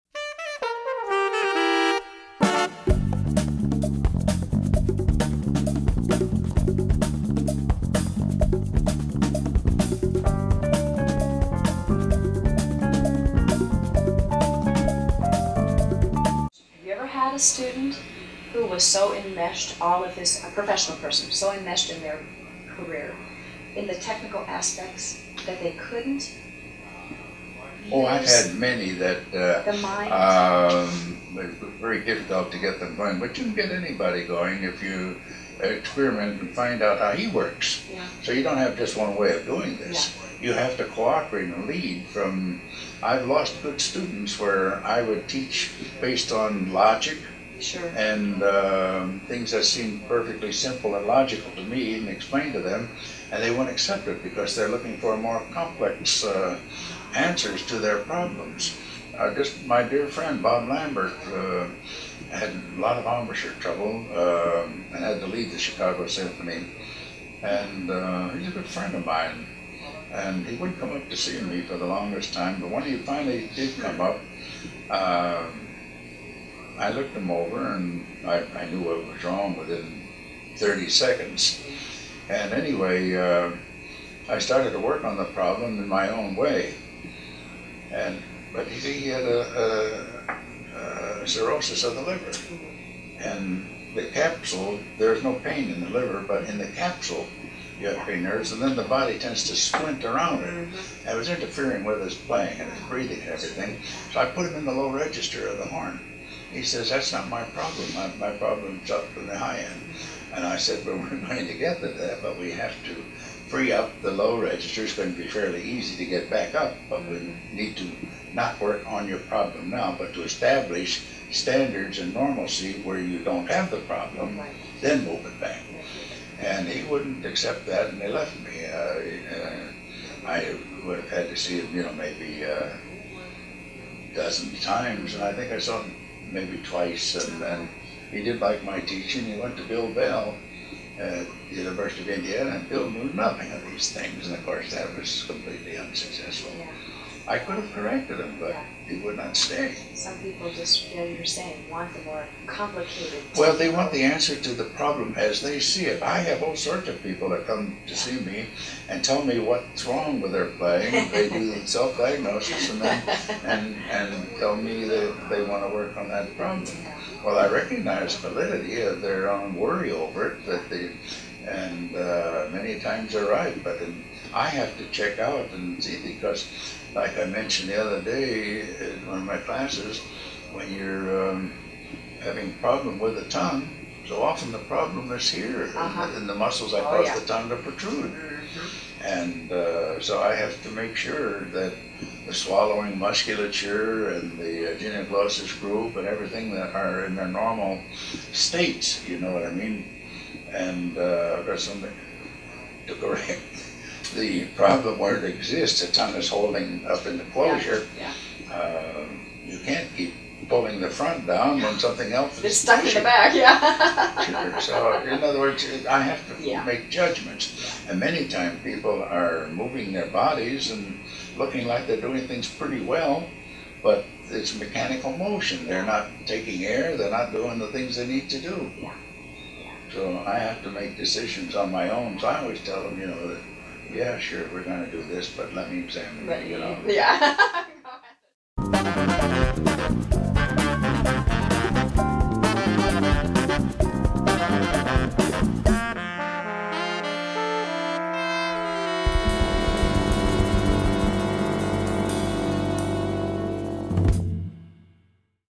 TPTV - Arnold Jacobs: Finding Out How the Student Works
Arnold Jacobs described the process of determining what motivates a student. Jacobs observed that he had lost many students who did not align with his way of teaching. He said it is an imperative to search out how what excites the student, what interests them. Sometimes a student will be reacting to and focused on the symptom rather than the root cause. This can be a negative distraction for the student, and cause issues as the teacher attempts to address the problem.